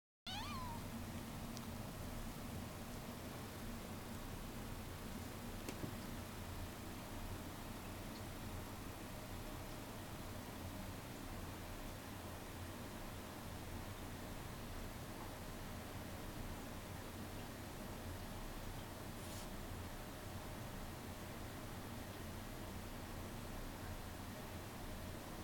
bagの中が好きな愛猫
いなくなったと思っていたらバッグの中に入っていました。